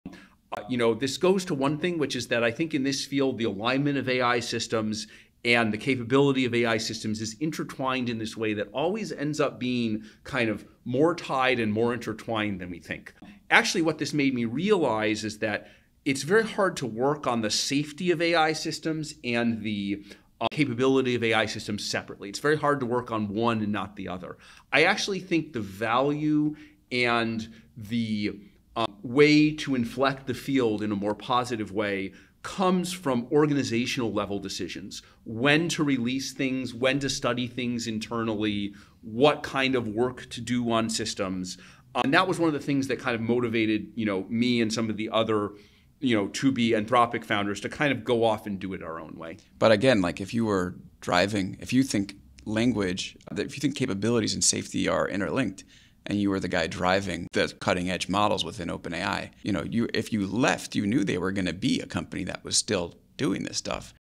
AI Safety & Capability: Why They're Linked & What Matters Most
Safety and capability: As models cross capability thresholds, should requirements tighten in lockstep? Comment the one test that matters most."